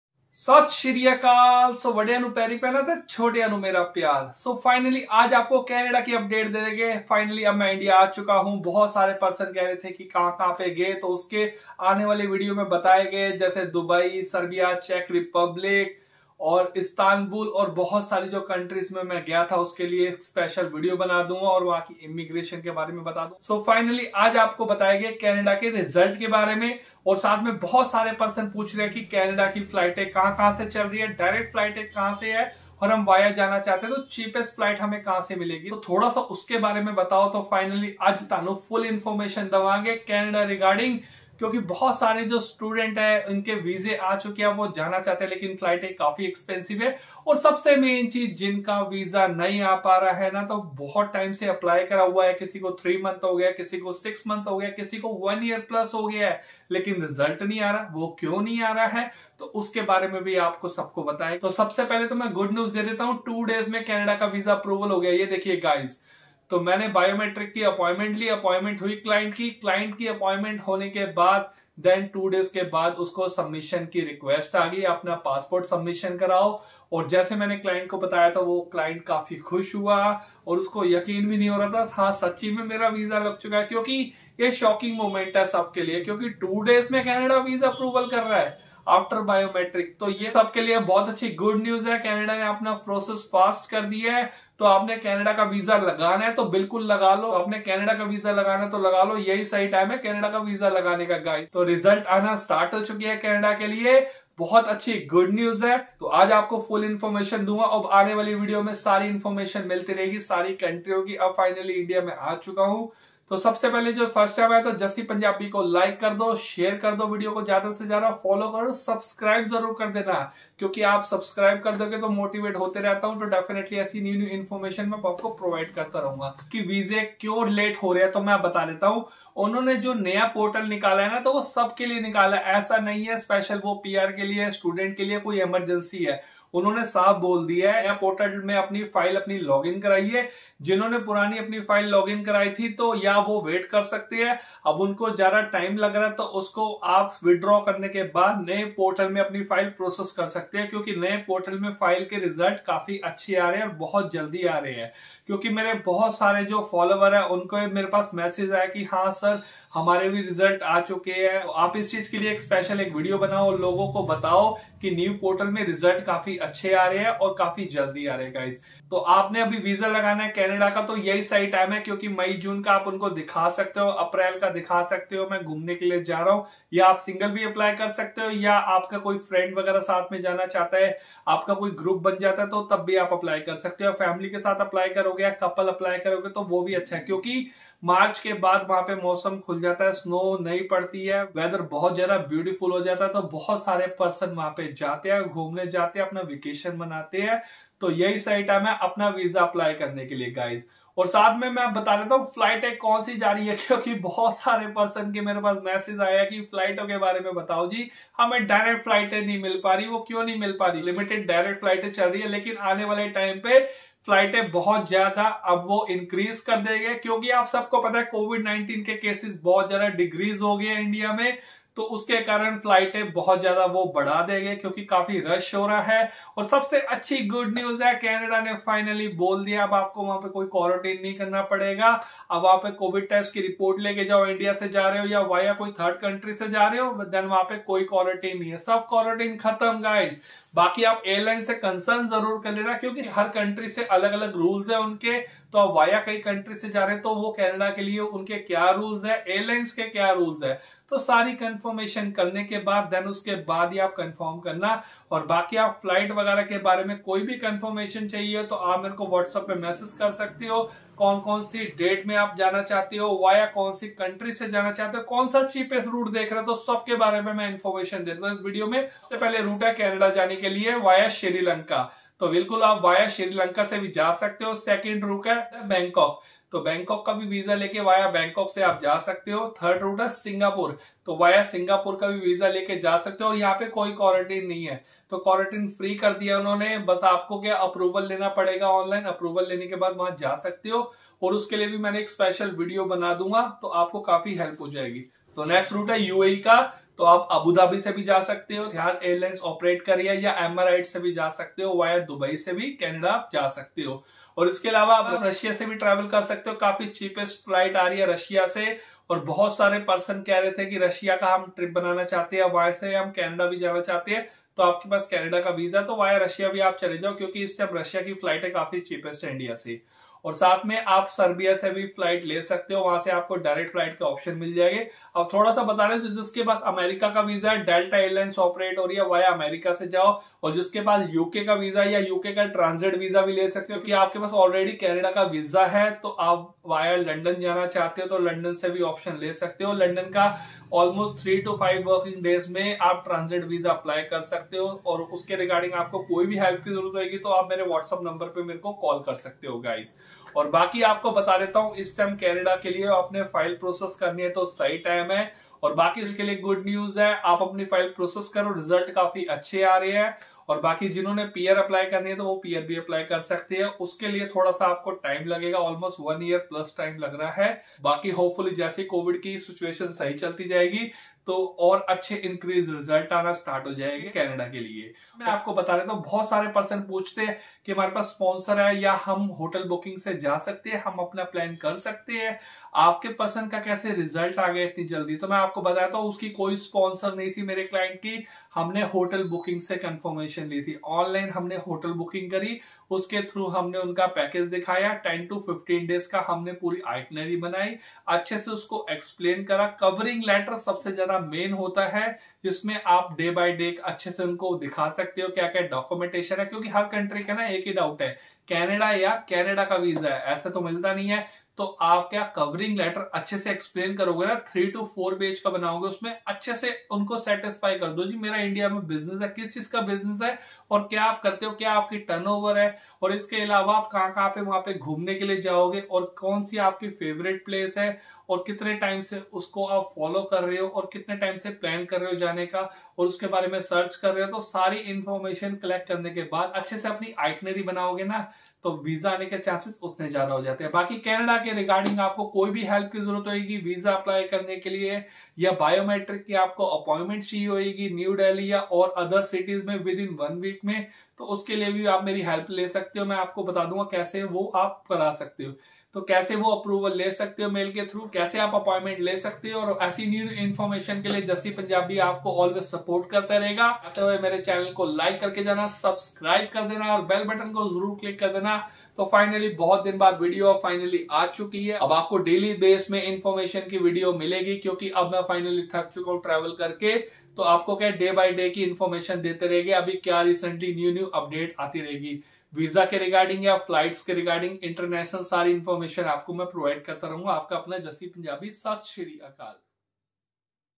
CANADA VISA LATEST NEWS | VISA APPROVED IN 2 DAYS | INDIA TO CANADA DIRECT FLIGHT NEWS | VFS UPDATE
VFS VISA OPEN | INTERNATIONAL TRAVEL REOPEN
OPEN | TOURIST VISA OPEN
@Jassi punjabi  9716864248

He can join the group. @Jassi punjabi

FacebookPAGE -jassifoodie